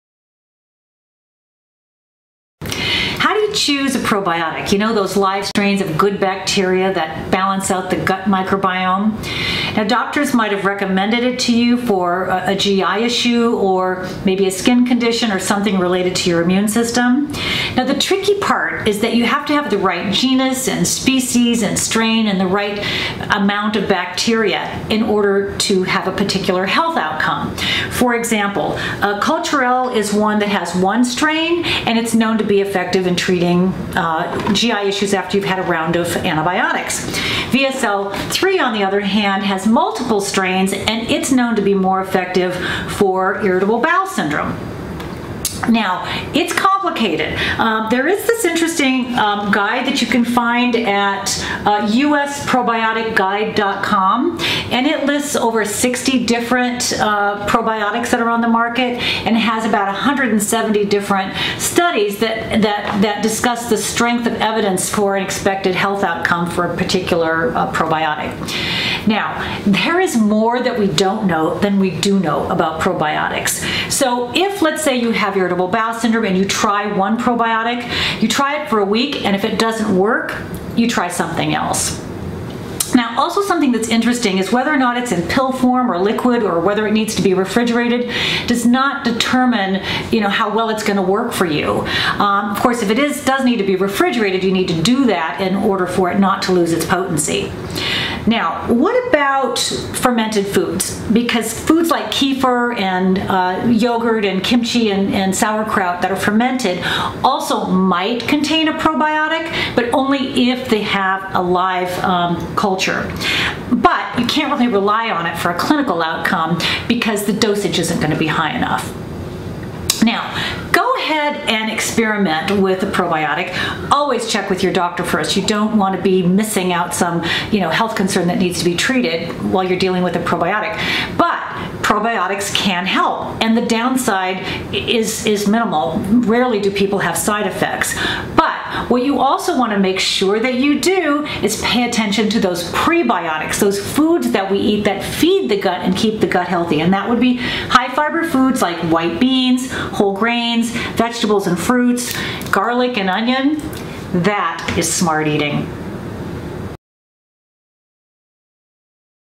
Thinking of adding a probiotic to your regimen? How to choose | Smart Eating Show
How do you choose a probiotic –

those supplements that contain live strains of “good bacteria”
that help balance the microbiome in your gut?

Doctors often recommend them to offset the side effects of antibiotics,
but they may also be used to treat various skin conditions,
irritable bowel syndrome and a host of other issues,
including immune issues which involve the gut.

But the tricky part is getting the right one.

You need the right strain, genus and species plus the right dosage to
make sure it is effective for a particular health concern.
For example,  Culturelle is a probiotic that has one strain of bacteria and has been shown to be effective to treat GI side effects associated with antibiotics.

VSL3 on the other hand has many strains, and seems to works better for IBS or ulcerative colitis.  It’s a bit more expensive and needs to be refrigerated.

For a list of 65 probiotics and references outlining the strength of scientific evidence for various expected health benefits, check out usprobioticguide.com.

And by the way, whether the probiotic comes as a pill, liquid or requires refrigeration does not necessarily determine its effectiveness for a particular concern.  It depends on the type of bacteria and how much.

When it comes to probiotics, there is more we don’t know than what we do know, so experts suggest you try a probiotic for a week – say if you have IBS, and if it doesn’t provide some relief to your symptoms, try another one.  Of course, always check with your doctor. You don’t want to treat a condition with a probiotic and ignore needed medical attention.

And don’t forget that your diet has a huge effect on your gut.

Fermented foods like yogurt, kefir or sauerkraut MIGHT contain a probiotic, but only IF they have live cultures, and if they DO have live cultures, it will be listed on the label.  But you can’t rely on these "medical foods" for a clinical effect since they aren’t likely to have enough live bacteria.

So experiment with probiotics if you need them, and at the same time, feed your gut PREBIOTICS - veggies, fruit, whole grains, legumes and fermented foods with plenty of greens – that’s smart eating!